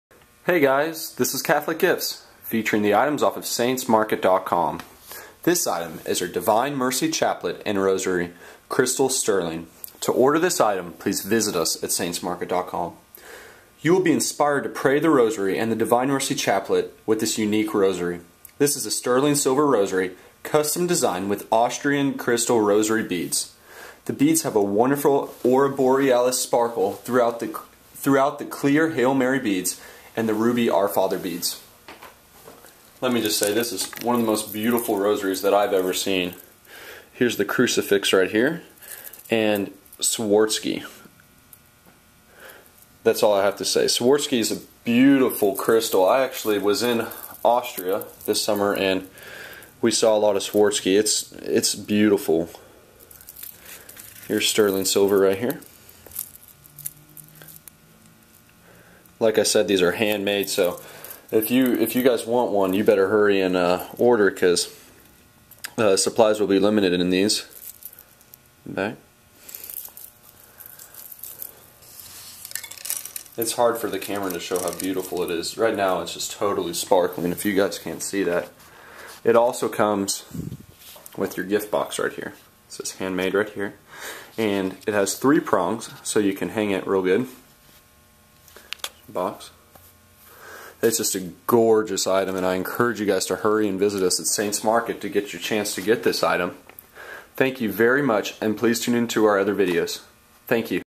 Buy Rosaries: Divine Mercy Chaplet ~ Swarovski Sterling Rosary
To purchase the Divine Mercy Chaplet ~ Rosary, please visit us . This beautiful rosary is made of sterling and swarovski crystal beads. The image of the Divine Mercy of Jesus and Saint Maria Faustia are on the rosary centerpiece.